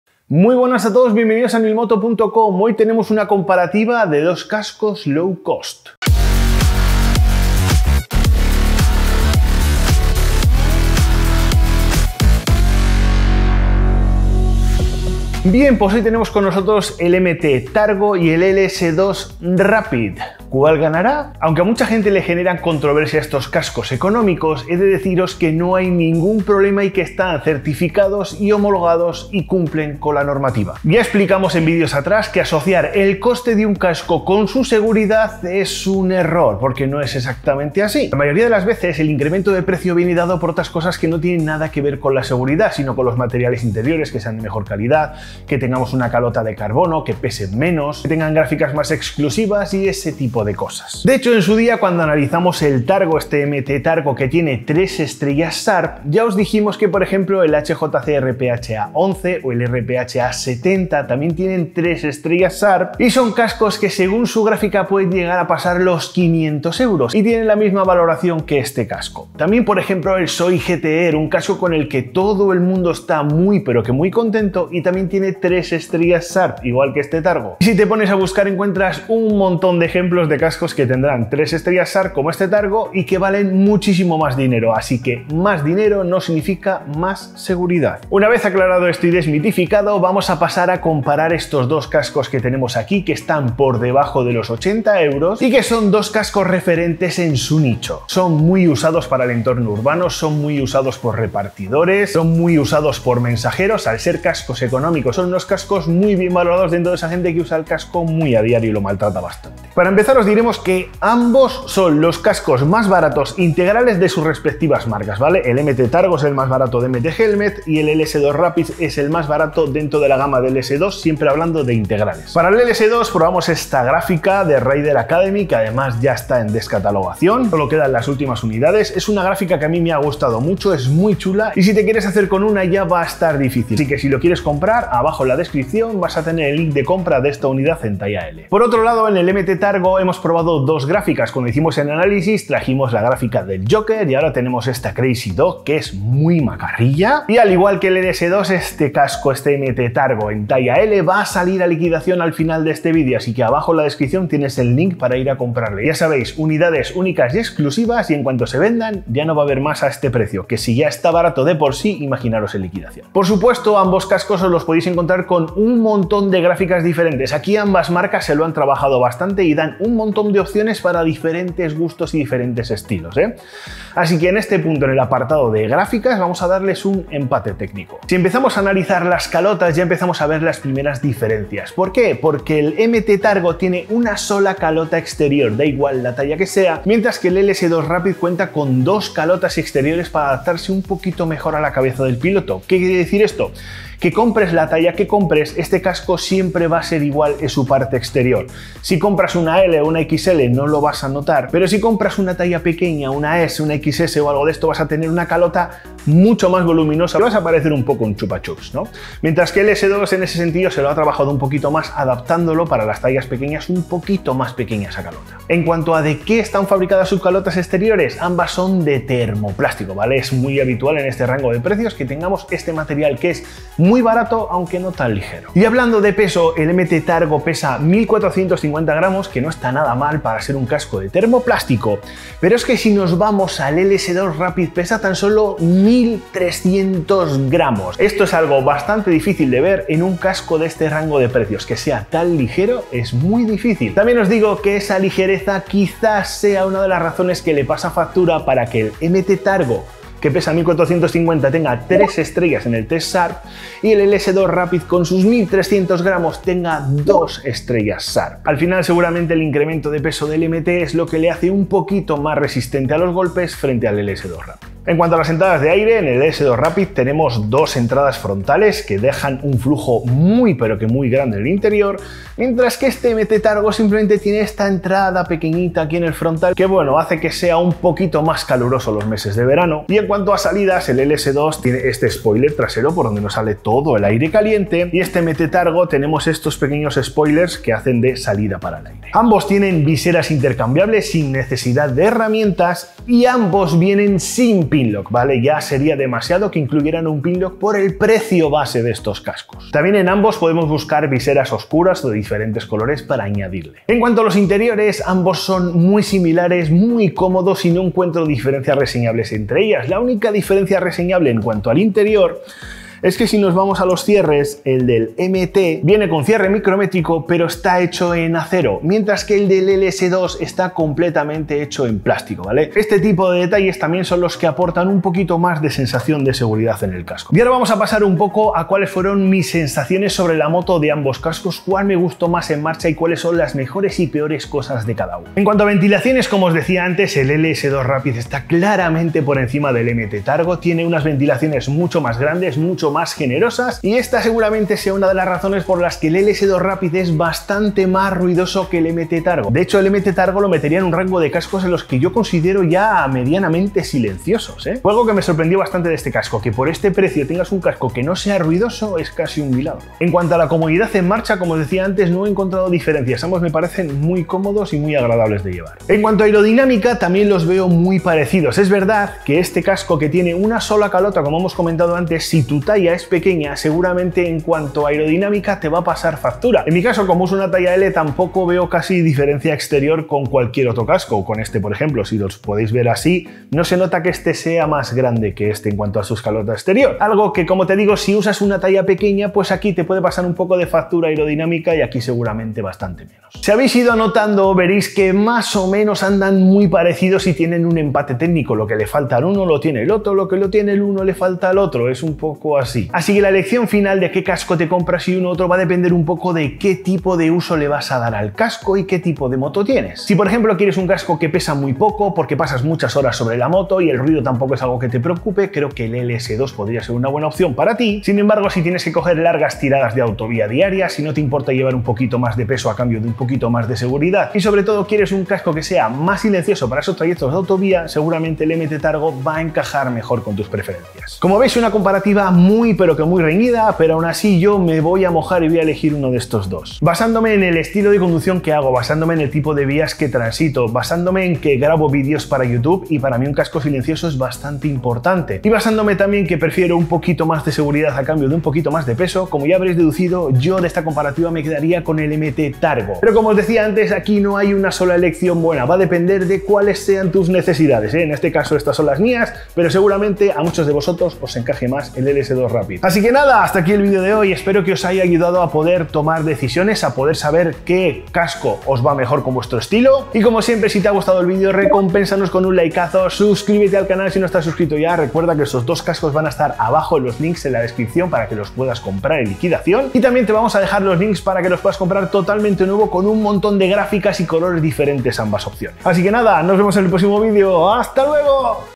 COMPARATIVA cascos de moto BARATOS. ( Me mojo y ELIJO uno) MT Targo Vs LS2 Rapid //Nilmoto
Hoy os traemos la comparativa de dos cascos low cost, el MT Helmets Targo y el LS2 Rapid. Cual ganará?